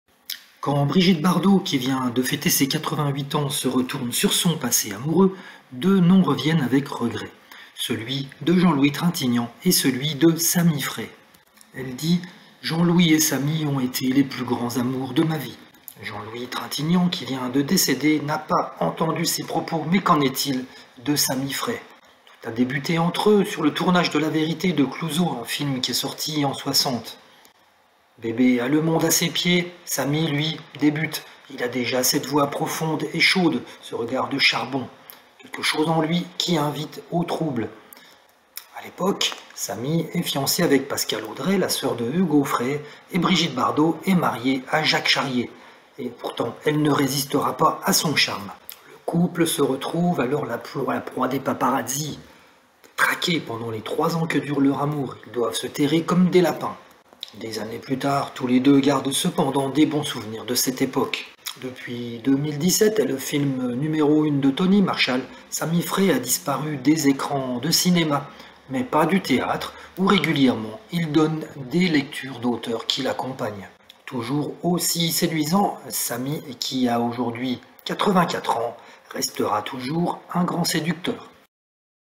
Brigitte Bardot a 88 ans : que devient Sami Frey, son autre grand amour avec JL Trintignant ?
Dans les années soixante, Brigitte Bardot, qui fête ce mercredi 28 septembre ses 88 ans, vivait une folle passion avec un débutant, Sami Frey, qui sera le séduisant David de Romy Schneider dans César et Rosalie.